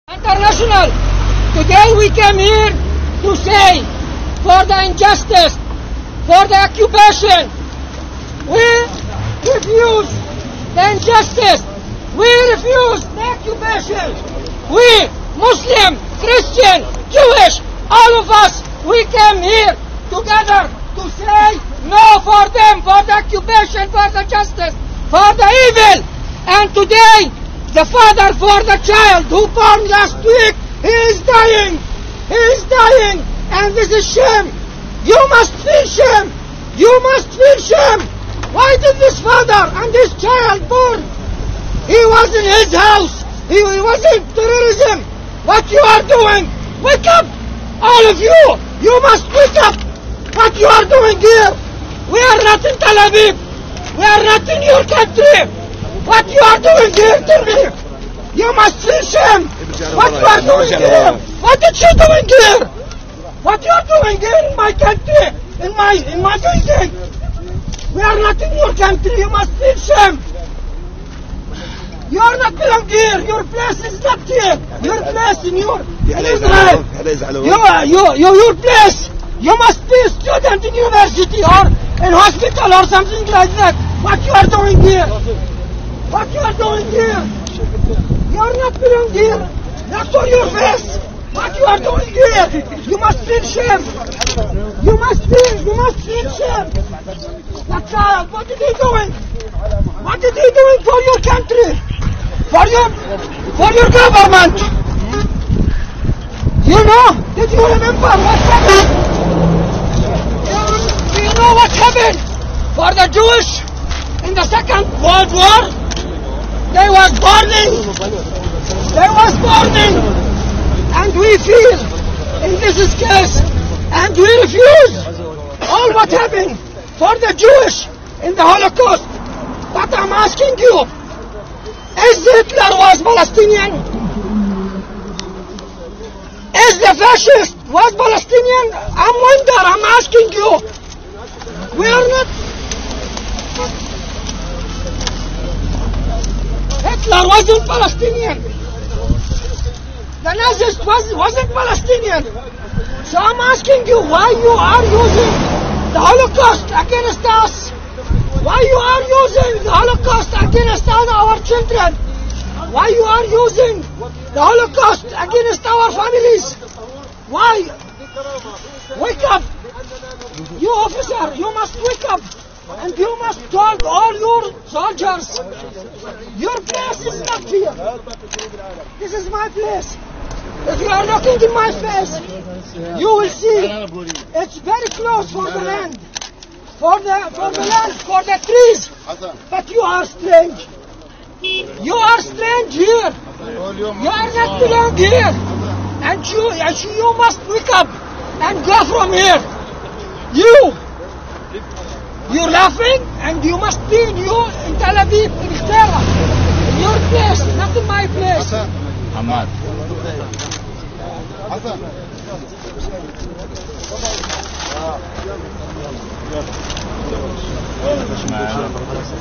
Bait All Baraka - South of Bethlehem
Hasan Breijieh give lessons for the israelian army